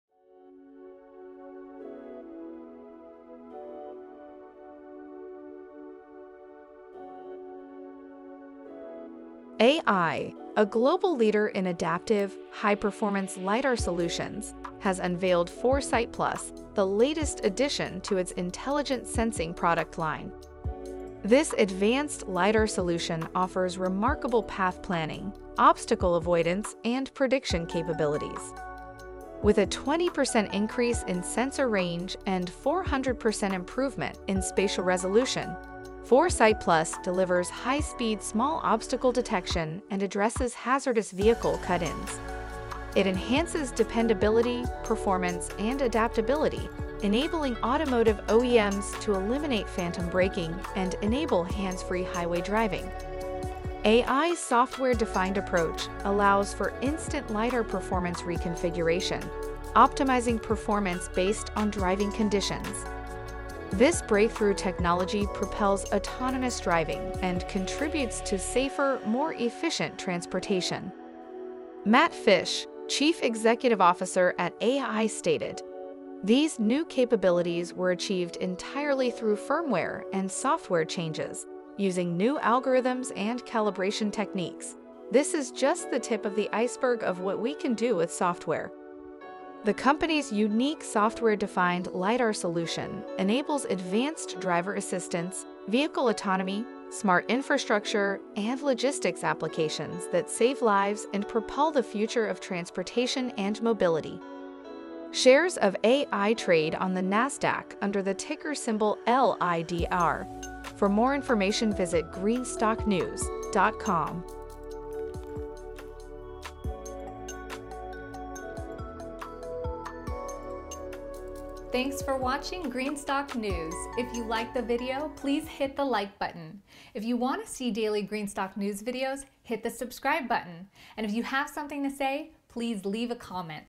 AEye's ($LIDR) 4Sight+ Elevates Lidar Performance for Advanced Path-Planning and Obstacle Avoidance
AEye, Inc. (NASDAQ: LIDR), a global leader in adaptive, high-performance lidar solutions, has unveiled 4Sight+, the latest addition to its Intelligent Sensing product line.

This advanced lidar solution offers remarkable path-planning, obstacle avoidance, and prediction capabilities. With a 20% increase in sensor range and 400% improvement in spatial resolution, 4Sight+ delivers high-speed small obstacle detection and addresses hazardous vehicle cut-ins. It enhances dependability, performance, and adaptability, enabling automotive OEMs to eliminate phantom braking and enable hands-free highway driving.

AEye's software-defined approach allows for instant lidar performance reconfiguration, optimizing performance based on driving conditions. This breakthrough technology propels autonomous driving and contributes to safer, more efficient transportation.

Matt Fisch, Chief Executive Officer at AEye stated:“These new capabilities were achieved entirely through firmware and software changes, using new algorithms and calibration techniques; this is just the tip of the iceberg of what we can do with software.”

AEye’s unique software-defined lidar solution enables advanced driver-assistance, vehicle autonomy, smart infrastructure, and logistics applications that save lives and propel the future of transportation and mobility.

Shares of AEye trade on the NASDAQ under the ticker symbol LIDR.